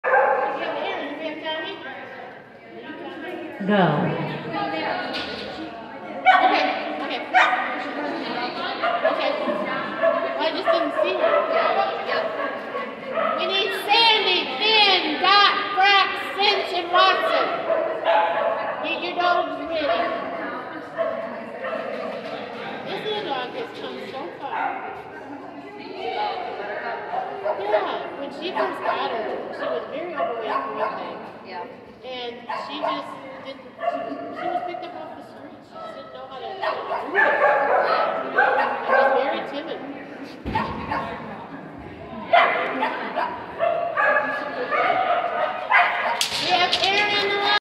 Colors 4/10/2022 Level five
a great run for buttercup! Had to listen to the background conversations were having me tear up. it's awesome to know that others notice how far we have come!